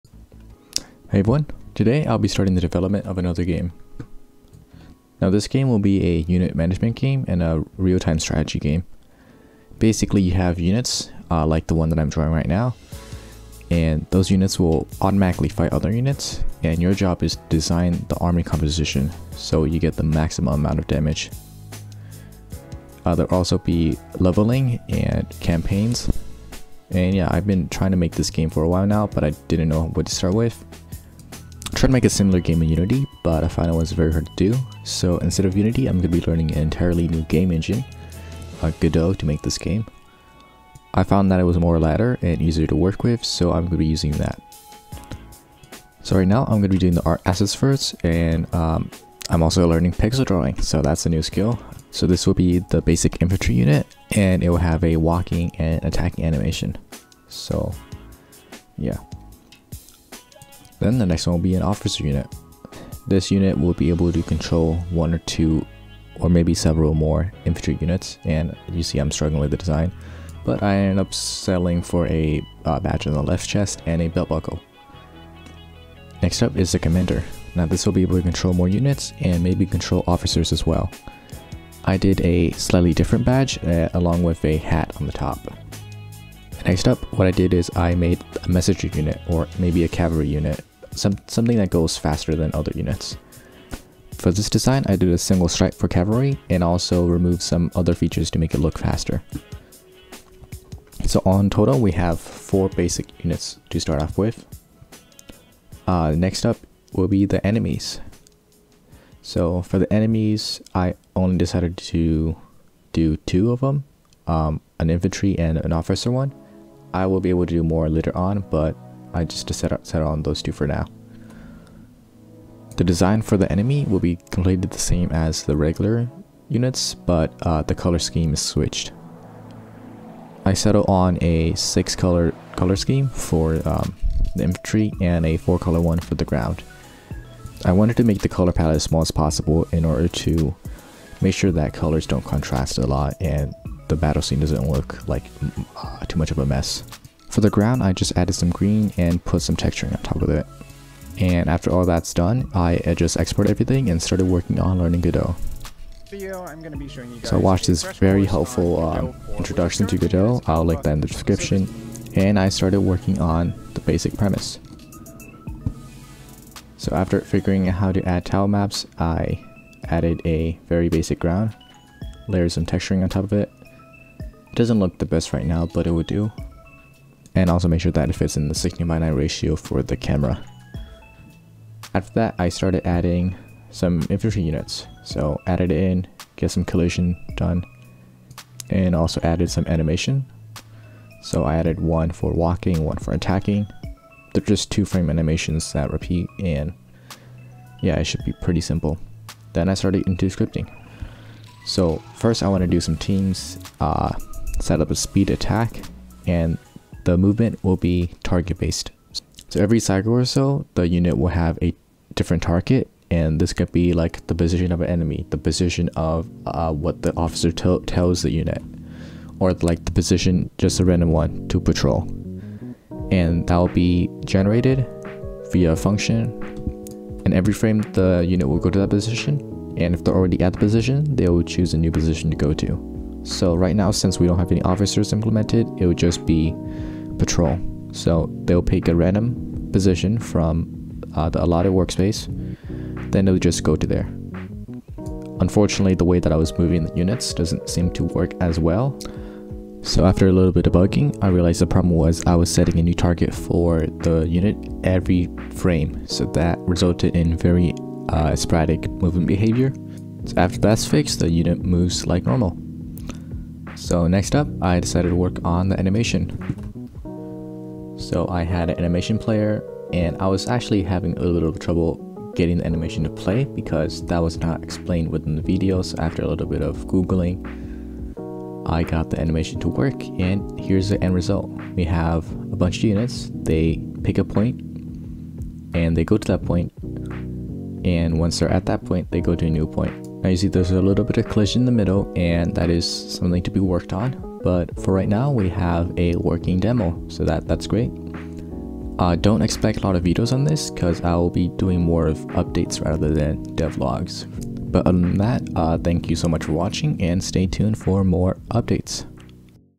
Creating EPIC Strategy Games in Godot 4: Creating Art and Basic Pathfinding | Dev Log #1
Today I started the development of a real time strategy game where you manage units. It'll have leveling, combat and AI within it by the end.